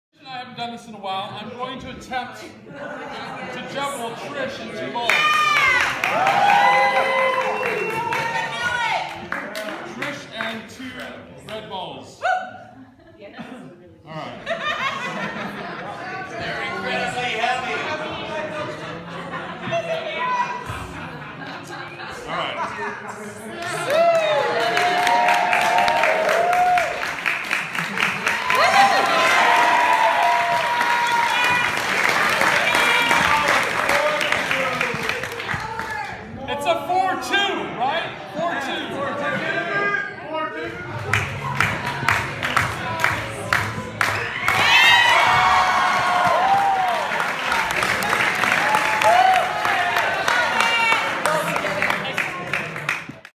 HJF 2017 Walter Juggles Trish
Walter "juggles" Trish at Humboldt Juggling Festival Renegade.